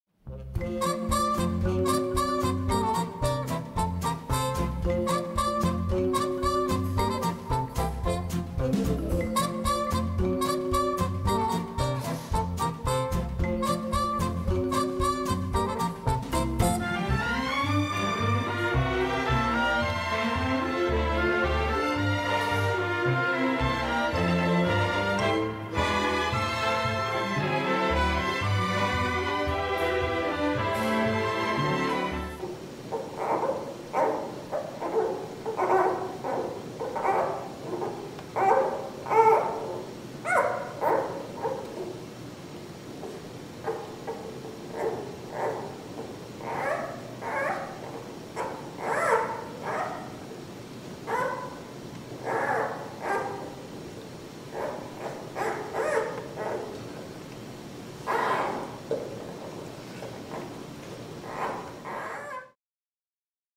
Süße Laute aus dem Zoo Berlin: So klingen Panda-Babys
Die beiden Panda-Zwillige im Berliner Zoo entwickeln sich prächtig. Der Zoo hat nun eine Tondatei von ihren Lauten veröffentlicht.